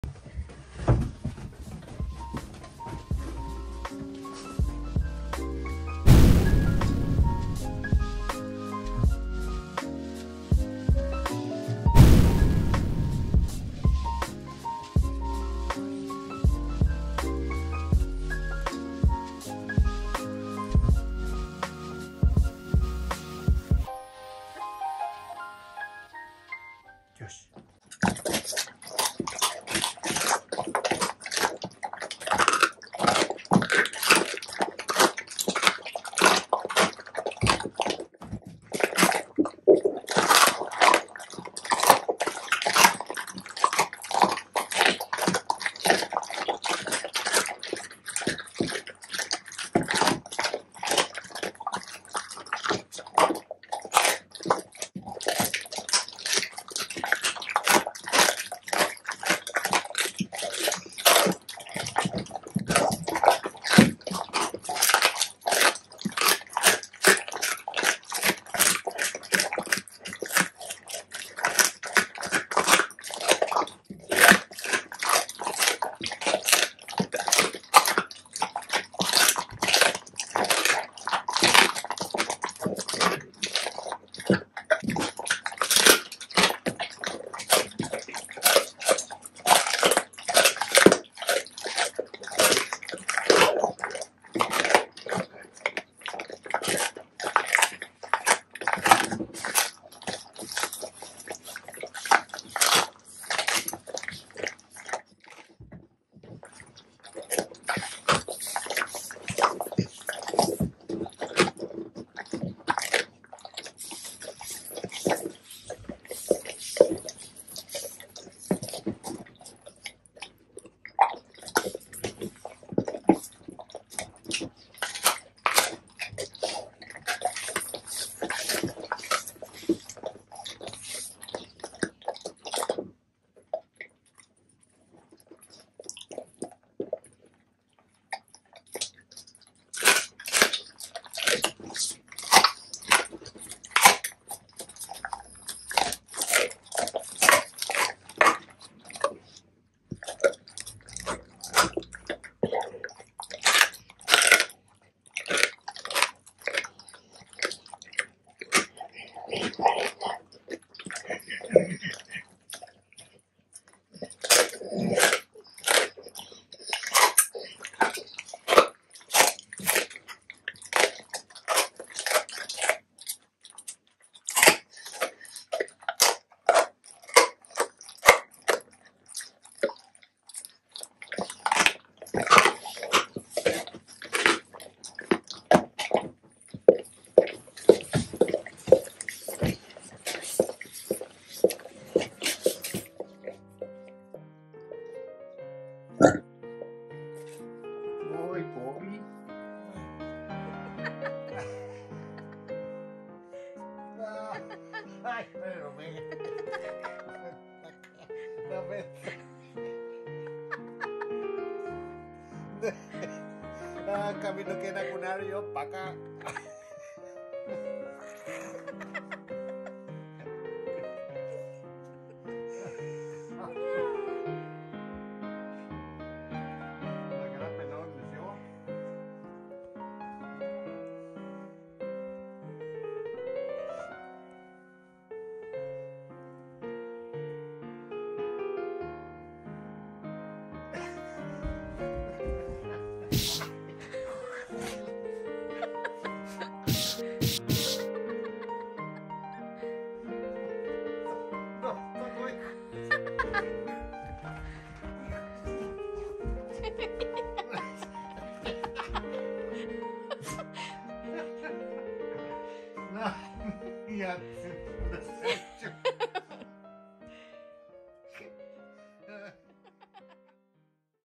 【ASMR】 Dogs eating Raw Meats
🖤Black Labrador🖤TICO ♂ 2017.9.8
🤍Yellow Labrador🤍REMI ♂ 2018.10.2